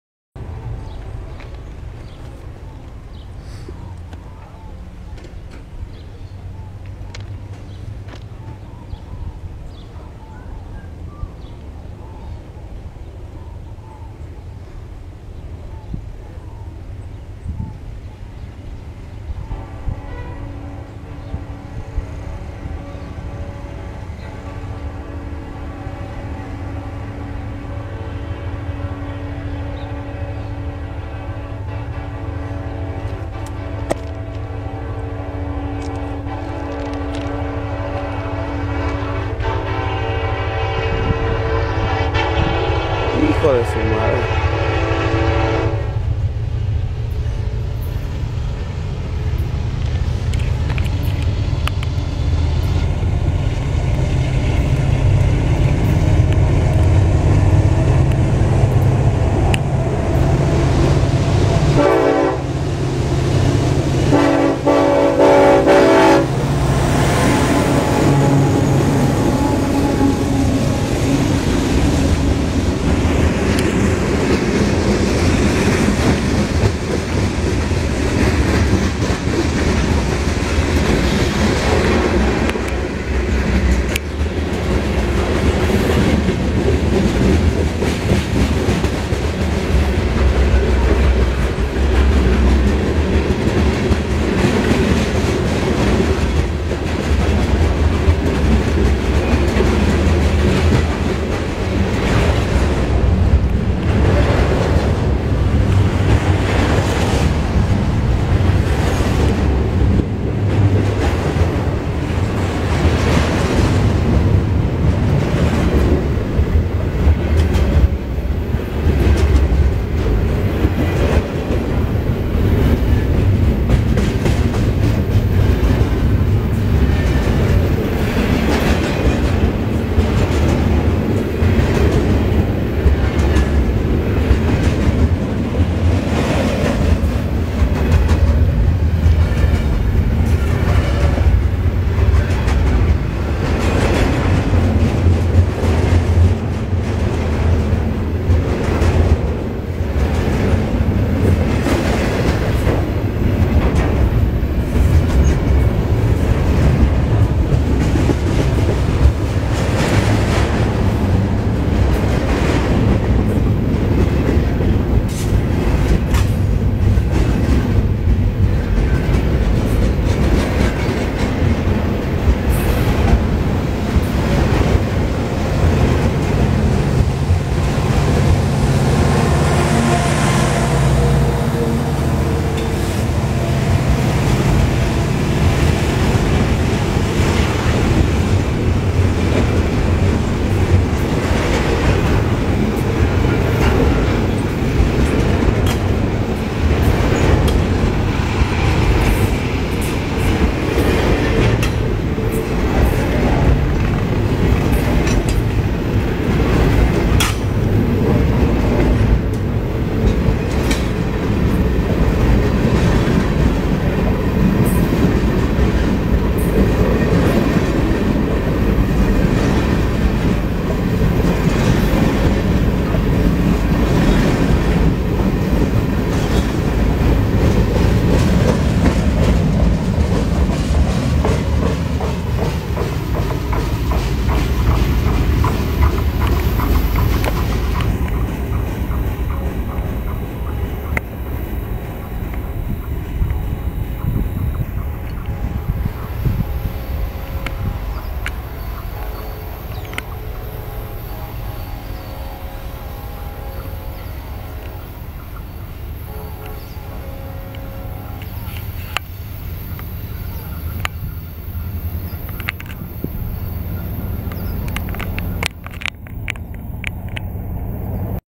casi se los lleva el tren
por poco se los lleva el tren, una camioneta